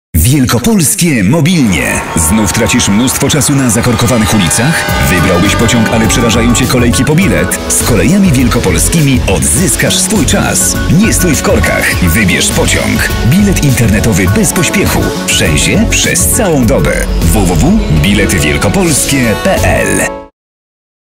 spot reklamowy - bilet internetowy
Wielkopolskie Mobilnie!
Znów tracisz mnóstwo czasu na zakorkowanych ulicach?
Wybrałbyś pociąg ale przerażają cię kolejki po bilet?
Z Kolejami Wielkopolskimi Odzyskasz swój czas! Nie stój w korkach, wybierz pociąg!
Bilet internetowy bez pośpiechu! –wszędzie – przez całą dobę!